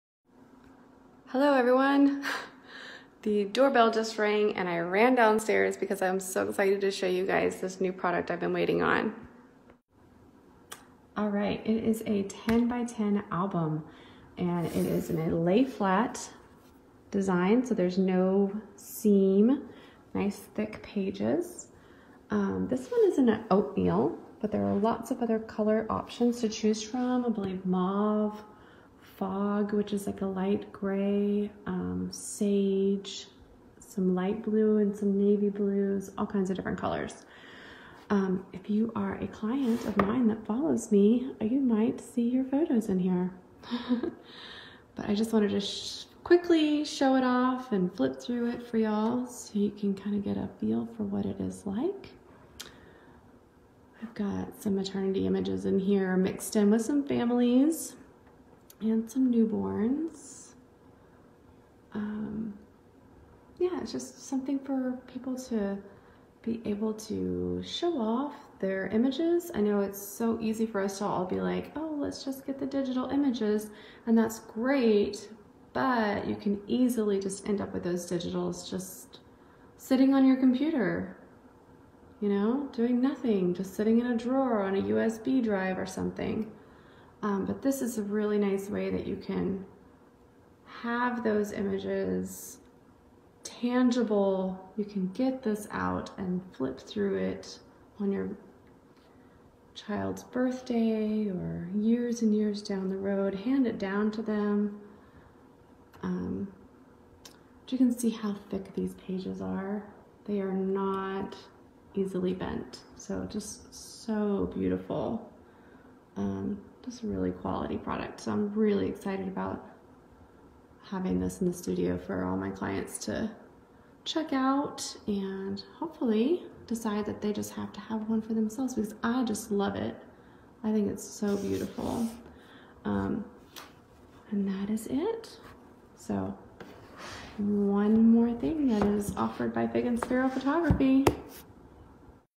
Album Sample
The 10x10 album with the Oatmeal cover.  This album is in a lay-flat design, with smooth matte fine art paper.  It is one of my very favorite ways to display images from your session.  I am so excited to be offering this beautiful piece of art to my clients.